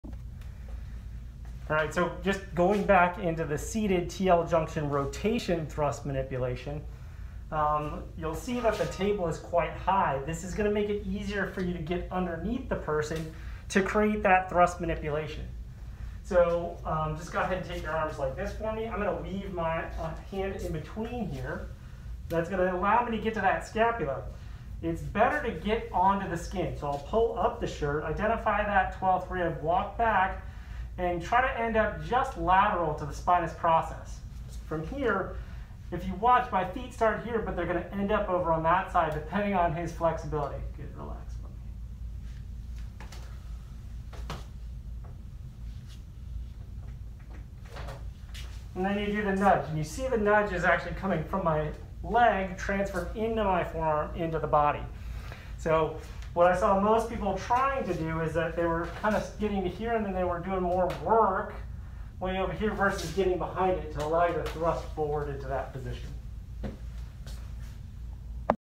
Footwork for the seating thoracolumbar rotational thrust manipulation
These videos support educational training provided to students through in-person courses delivered by Kevin MacPherson PT, DPT, FAAOMPT. Do not perform this technique without proper education and training.